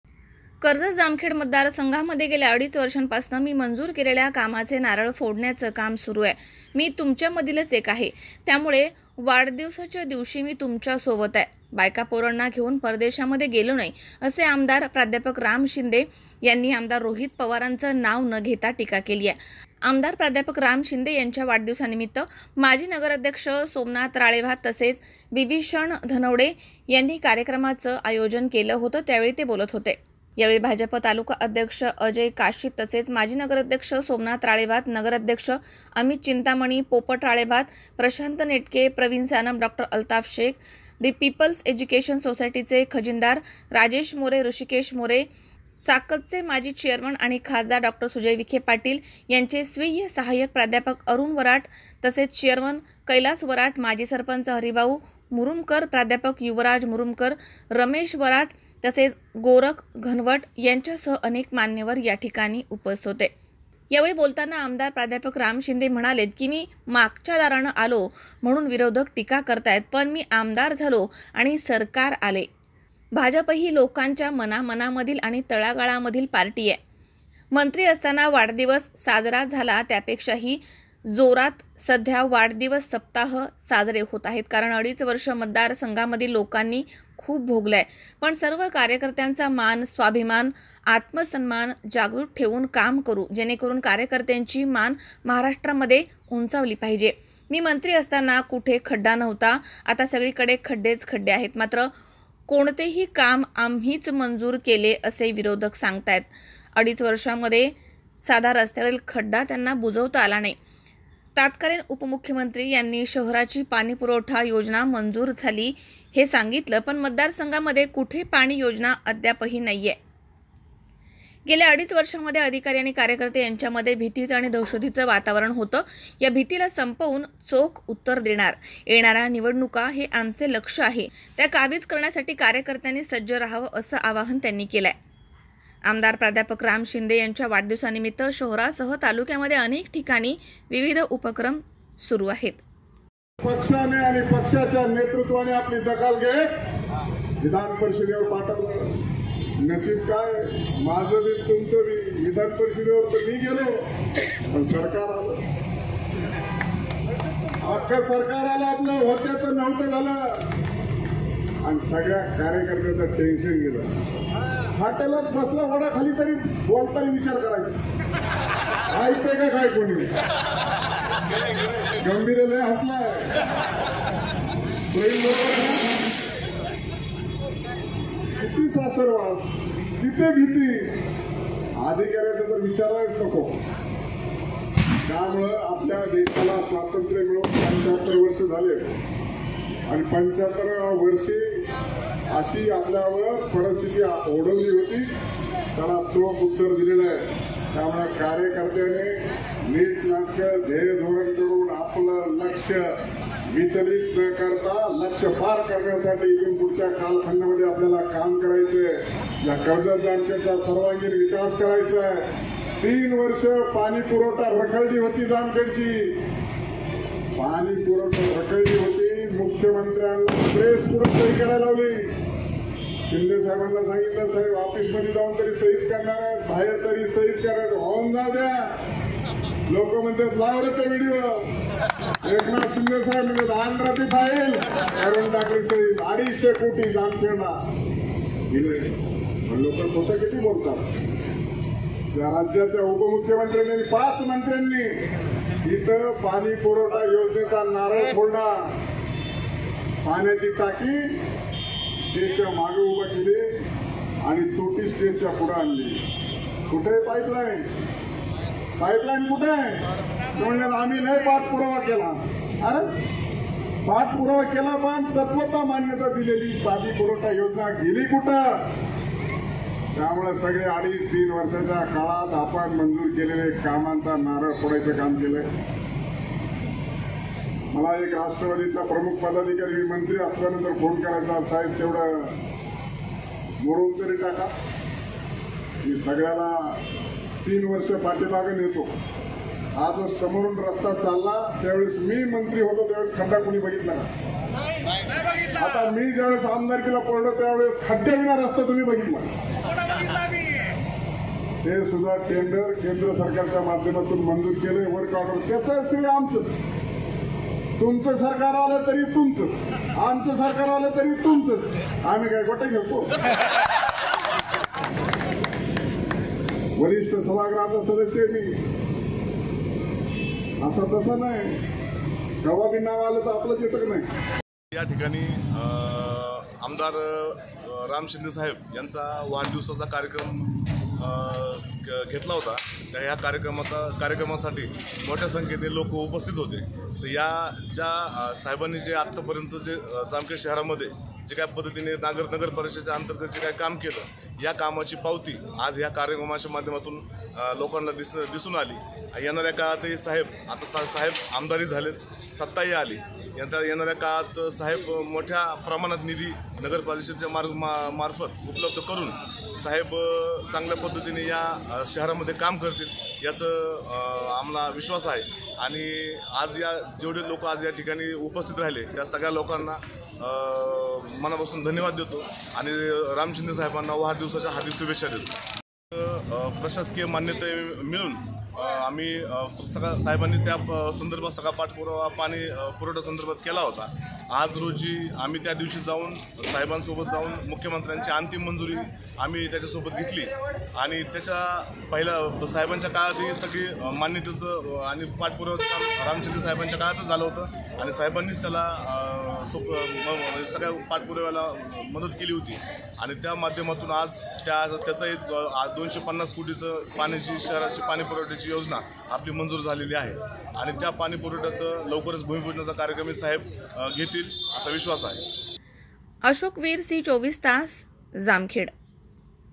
मी मंत्री असताना कुठेही खडे नव्हते अता सगळी कडे खड्डेच खडे - आमदार प्रा. राम शिंदे !
कर्जत - जामखेड मतदारसंघात गेल्या अडीच वर्षांपासून मी मंजूर केलेल्या कामाचे नारळ फोडण्याचे काम सुरू आहे. मी तुमच्यातील आहे त्यामुळे वाढदिवसादिवशी तुमच्या सोबतच आहे. बायका पोरांना घेऊन परदेशात गेलो नाही. असे आमदार प्रा. राम शिंदे यांनी आमदार रोहित पवारांचे नाव न घेता टिका केली.  आमदार प्रा. राम शिंदे यांच्या वाढदिवसानिमित्त माजी नगराध्यक्ष सोमनाथ राळेभात, बिबिषन धनवडे यांनी कार्यक्रमाचे आयोजन केले होते यावेळी ते बोलत होते.